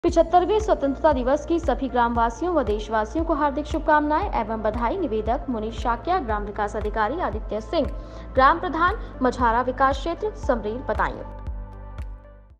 Aditya Singh (ग्राम प्रधान) मझारा विकास क्षेत्र समरेर Badaun की ओर से स्वतंत्रता दिवस की शुभकामनाएं
Munish Shakya (ग्राम विकास अधिकारी)